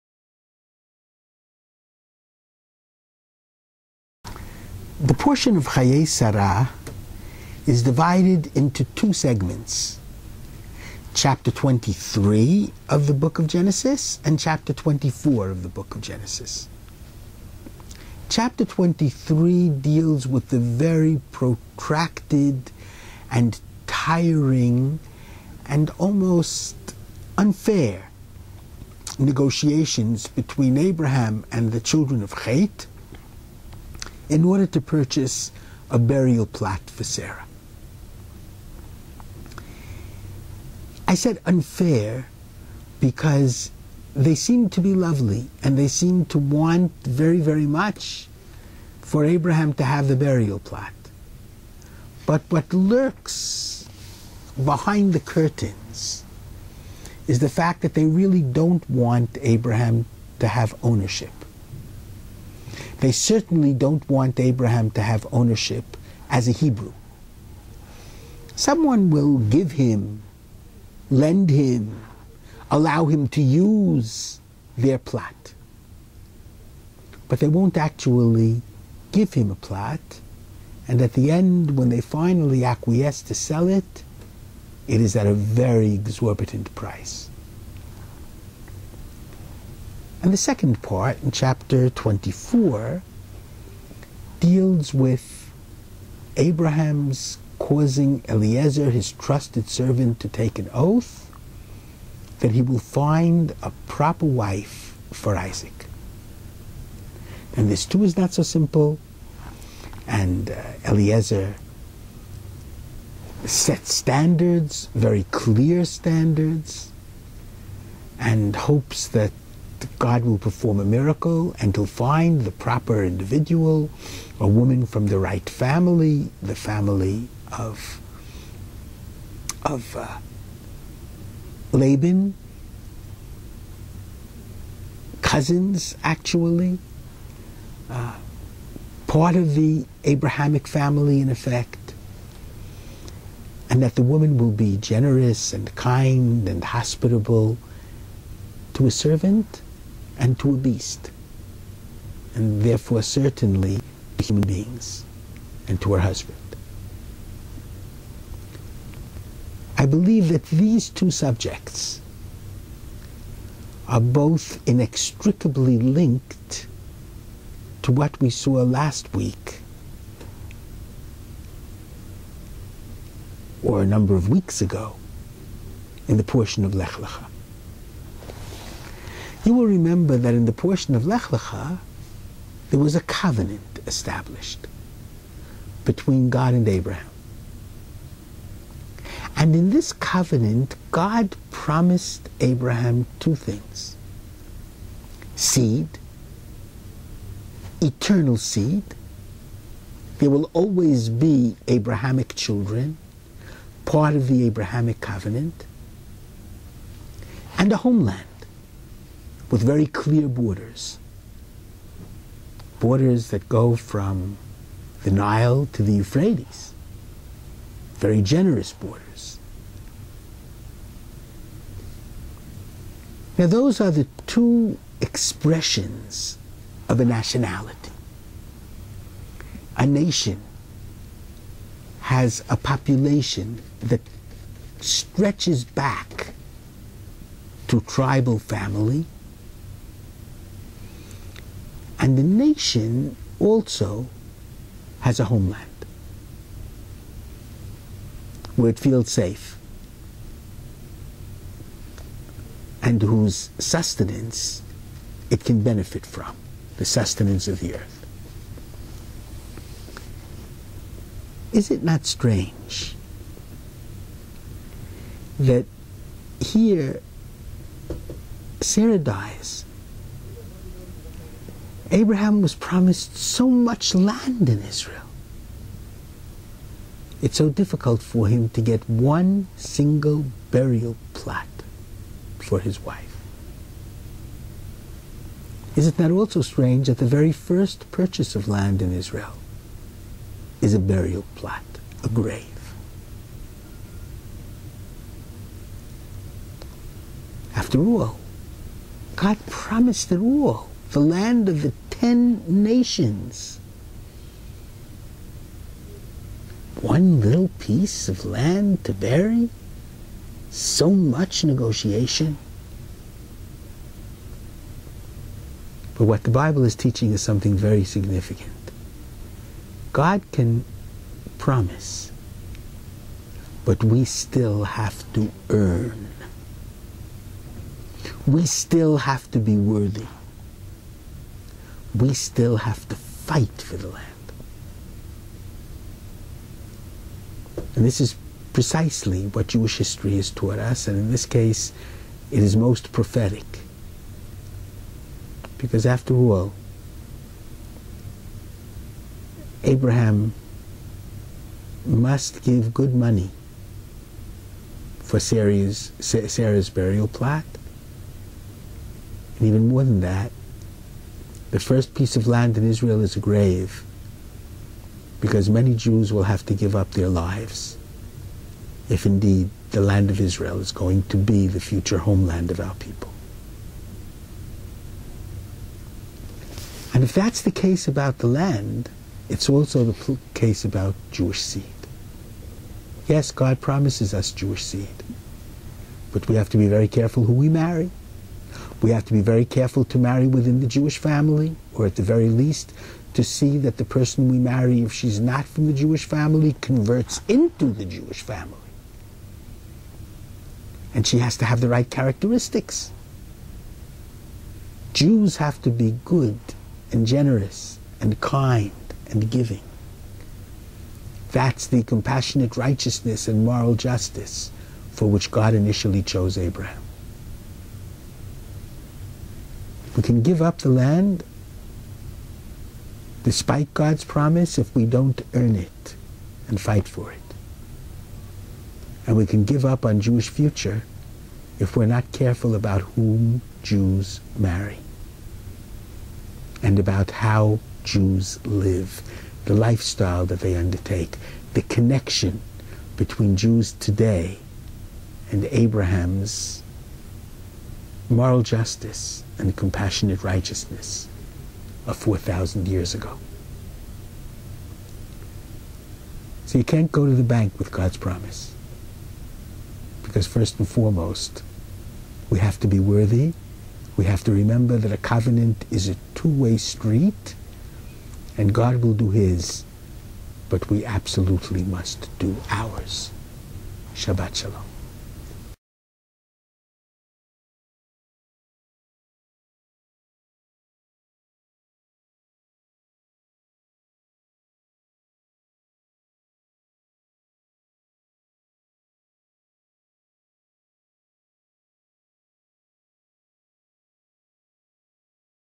Parshat Chayei Sarah - "Torah Lights" 5771
"Land and Seed: We Must Earn Them" -- Rabbi Shlomo Riskin's Insights into Parshat Chayei Sarah